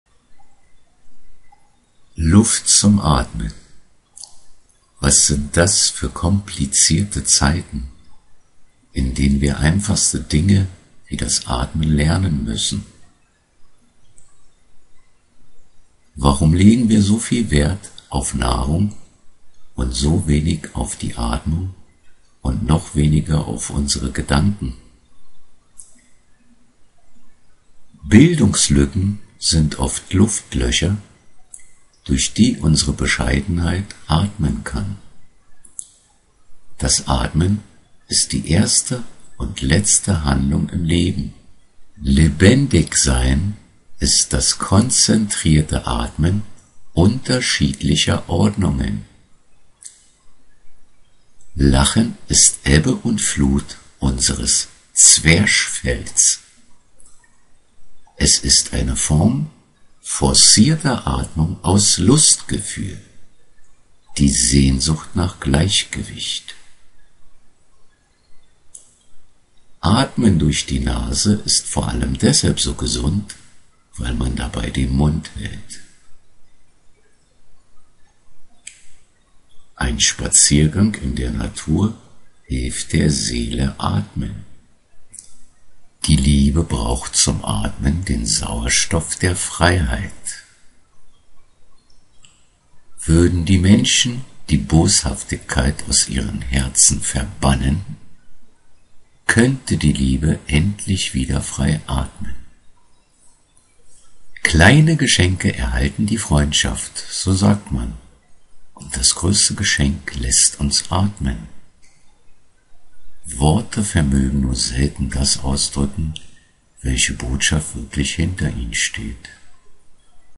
Luft zum Atmen
Herzlich Willkommen, gönne dir einen Moment Abstand vom Alltag!

Was sind das für komplizierte Zeiten, in denen wir einfachste Dinge wie das Atmen lernen müssen.
Warum legen wir so viel Wert auf Nahrung und so wenig auf die Atmung und noch weniger auf unsere Gedanken?
Bildungslücken sind oft Luftlöcher,
durch die unsere Bescheidenheit atmen kann.
Das Atmen ist die erste und letzte Handlung im Leben.
Lebendigsein ist das konzentrierte Atmen unterschiedlicher Ordnungen.
Lachen ist Ebbe und Flut unseres Zwerchfells. Es ist eine Form forcierter Atmung aus Lustgefühl: Die Sehnsucht nach Gleichgewicht.
Carl Ludwig Schleich
Atmen durch die Nase ist vor allem deshalb so gesund, weil man dabei den Mund hält.
Ein Spaziergang in der Natur hilft der Seele atmen.
Die Liebe braucht zum Atmen den Sauerstoff der Freiheit.
Würden die Menschen die Boshaftigkeit aus ihren Herzen verbannen, könnte die Liebe endlich wieder frei atmen.
Kleine Geschenke erhalten die Freundschaft, so sagt man, und das größte Geschenk lässt uns atmen.
Worte vermögen nur selten das ausdrücken, welche Botschaft wirklich hinter ihnen steht.
Der INFORMATIONCOACH sagt, die besten Ärzte der Welt sind:
Dr. Ruhe, Dr. Sinnvoll und Dr. Fröhlichkeit :-)
Heilungsmethoden Atemtrainer Coaching  Sam Rafea